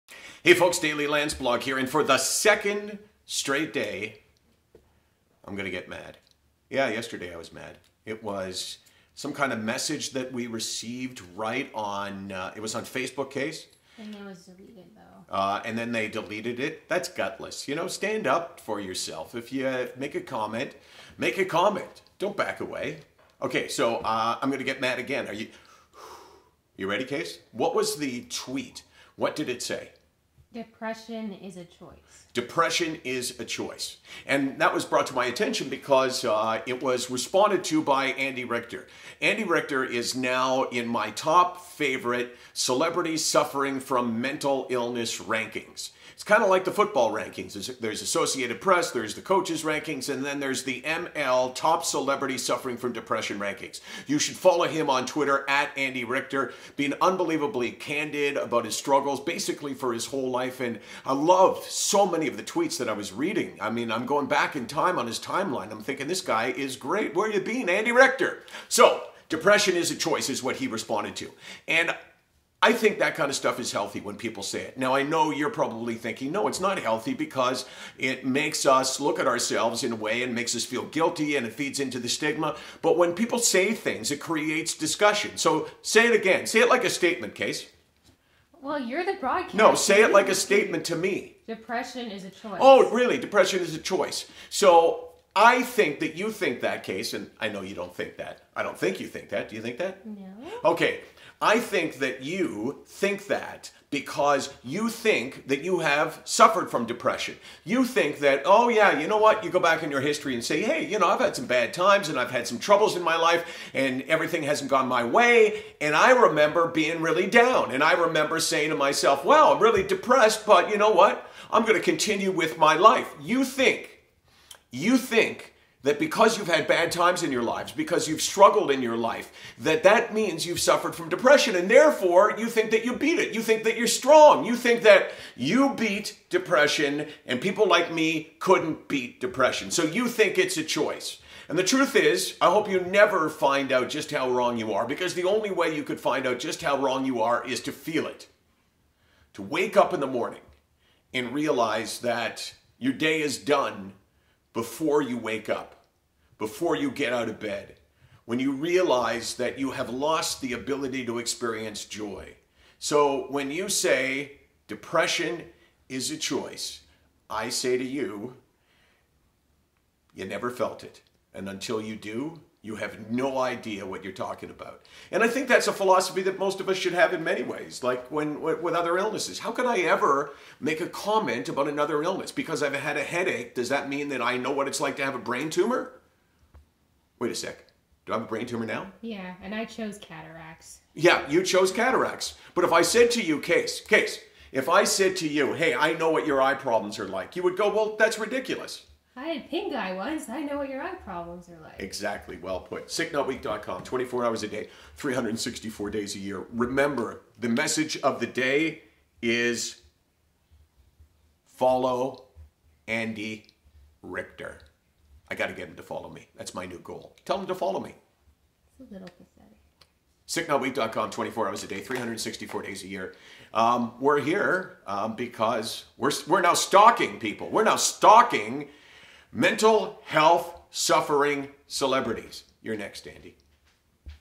Daily Landsblog: November 19, 2017
There are two takeaways from today's Landsblog.
1. Depression is not a choice. To anyone who says it is- we say you have never felt it. Please show this to that person in your life who has said "depression is a choice."
2. Michael has a mental health crush on Andy Richter. Go follow him on Twitter.